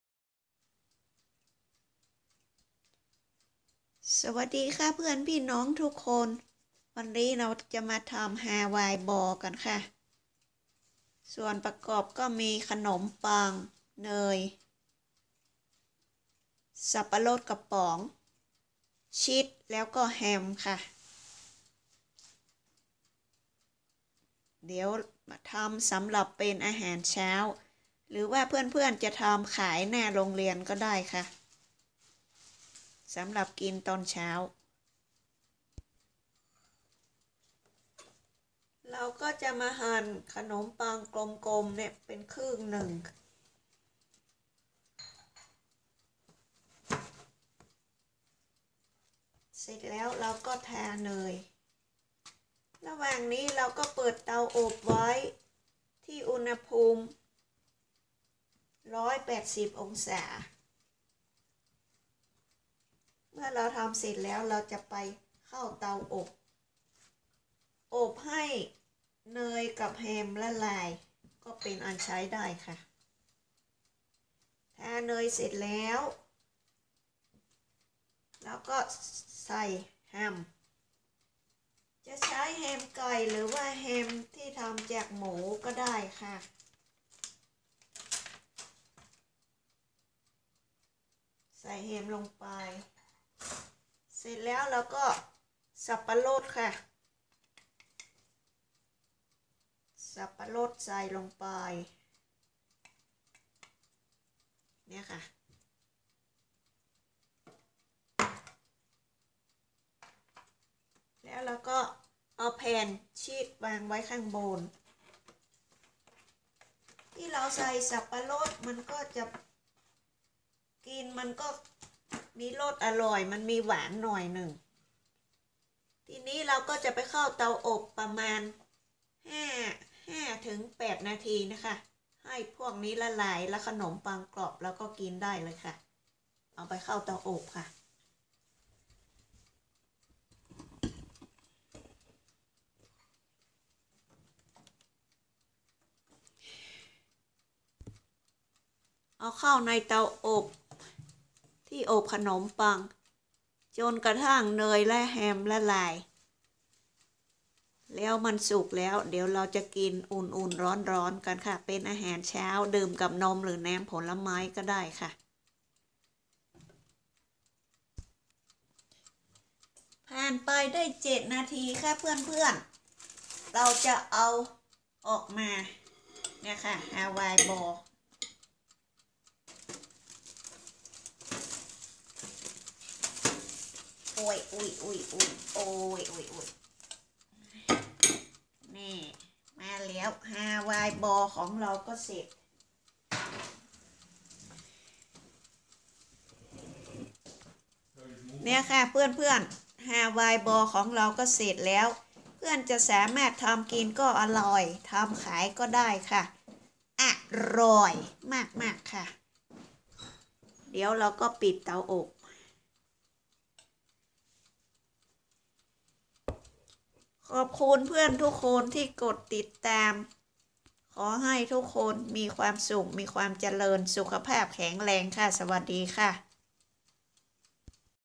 Hawai bol: ทำขนมปังฮาวายบอล ง่ายขายก็ได้
ขนมปังทำสะดวก  กินได้ทุกเวลา ใช้เวลาไม่นาน